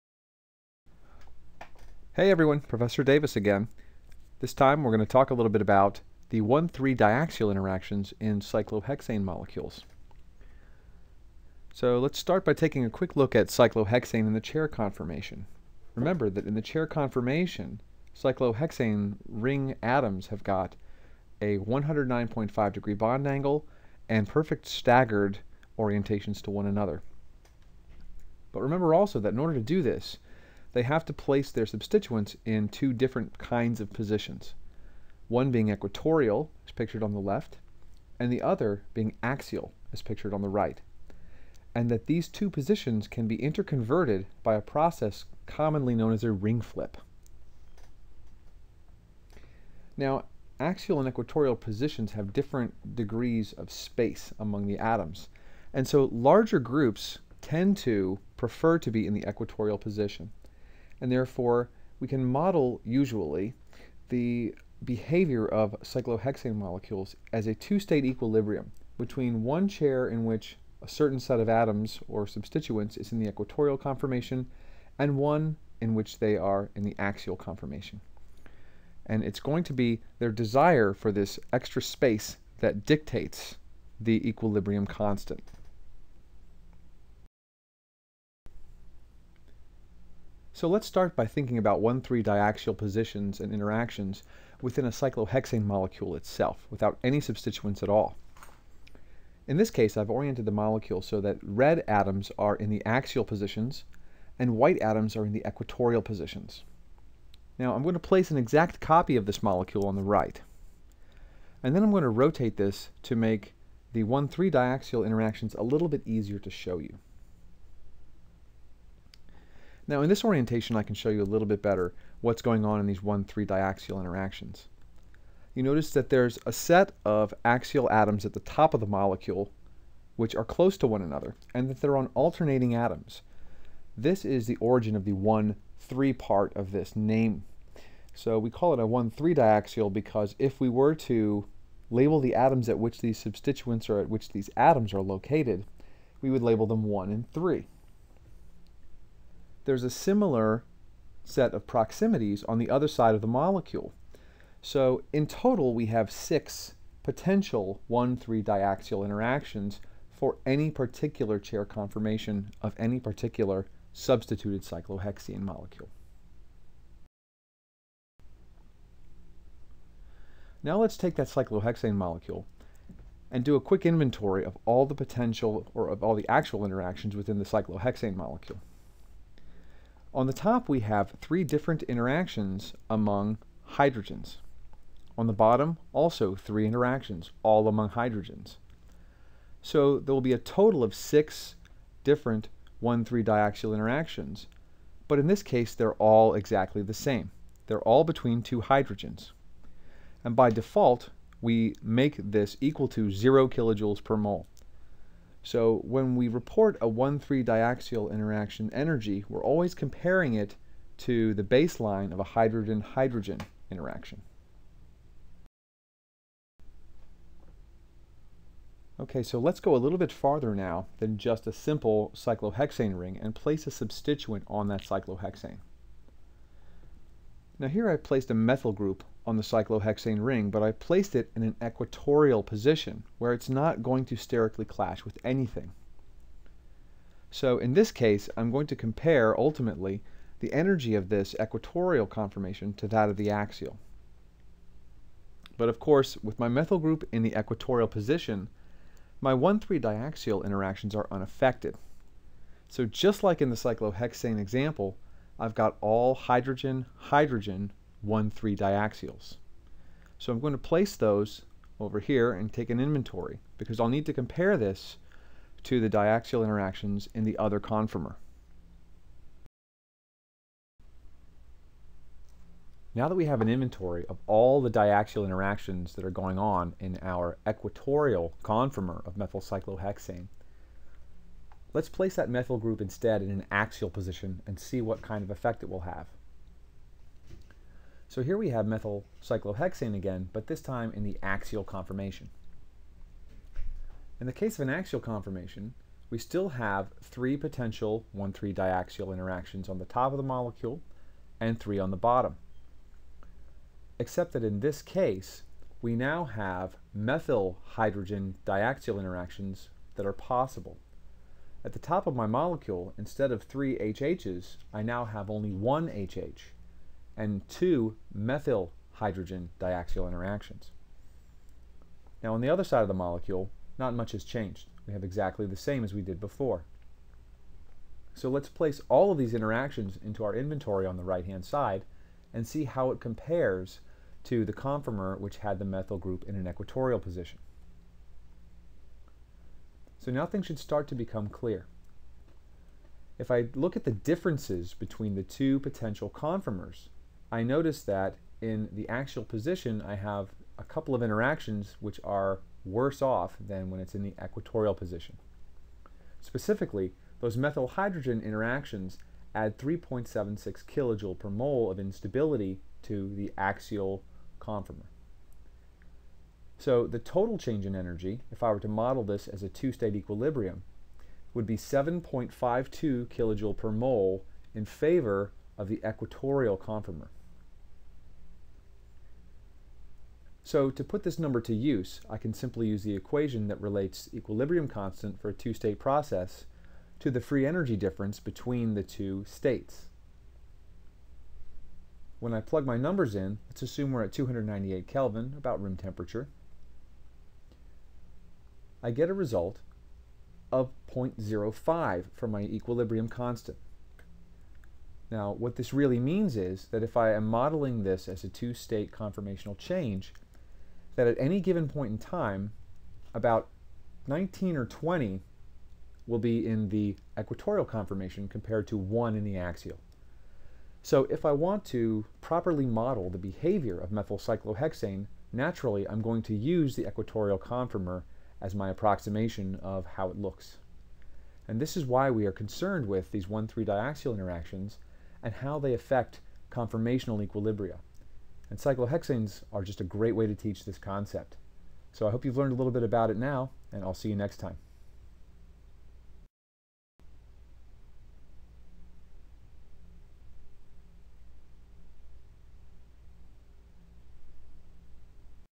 A brief introduction to 1,3-diaxial interactions
Professor Davis gives short introduction to diaxial interactions in cyclohexane.  These diaxial interactions can strongly affect conformational preferences (and therefore reactivity) of such compounds.  Cyclohexane and methylcyclohexane are both discussed.